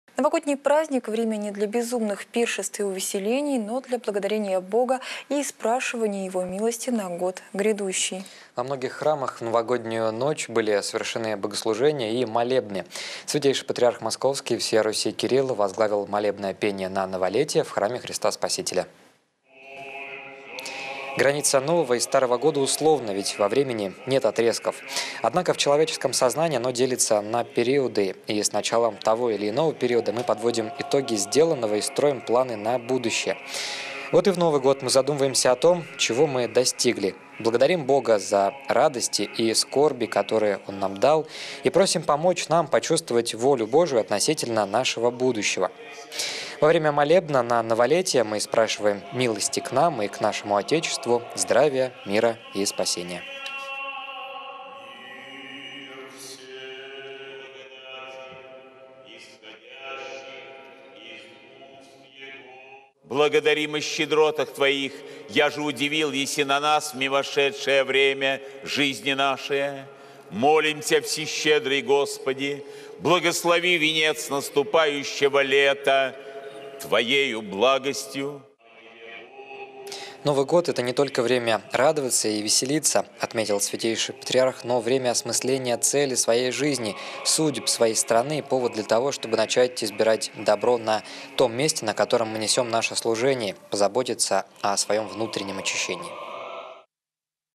Новолетие в Храме Христа Спасителя в Москве
Новогодний праздник – время не для безумных пиршеств и увеселений, но для благодарения Бога и испрашивания Его милости на год грядущий. Во многих храмах в новогоднюю ночь были совершены богослужения и молебны. Святейший Патриарх Московский и всея Руси Кирилл возглавил молебное пение на новолетие в Храме Христа Спасителя.